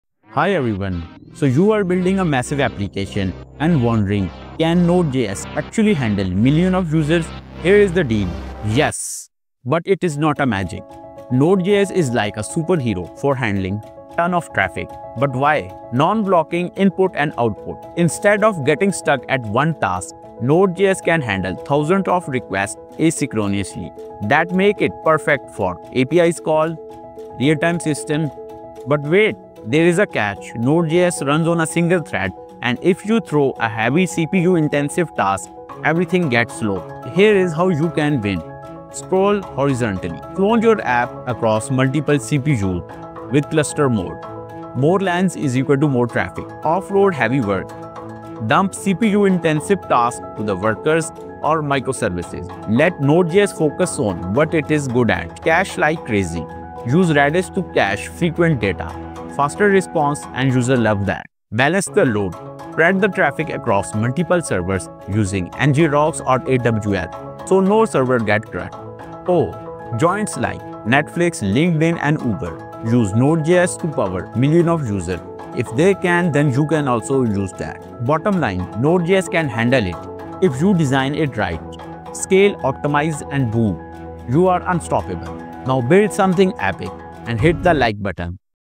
🔥Can Node.js Handle MILLIONS of Users? Scaling Secrets for High-Traffic Apps (Netflix Uber’s )
👋 Want to build a MASSIVE app with Node.js but worried about scalability? Discover how Node.js powers giants like Netflix, LinkedIn, and Uber to handle millions of users—and how YOU can do it too!

💡 In This Guide:
✅ Why Node.js’s non-blocking I/O is perfect for real-time apps (and its single-thread limitation)
✅ 4 PROVEN Scaling Strategies: Horizontal scaling, Worker Threads, Redis caching & Load Balancing
✅ How to avoid CPU bottlenecks & optimize performance
✅ Real-world case studies + architecture tips

🚀 Key Takeaways:

Clone apps across CPU cores with the Cluster module

Offload heavy tasks to microservices

Supercharge responses with Redis

Balance traffic using NGINX/AWS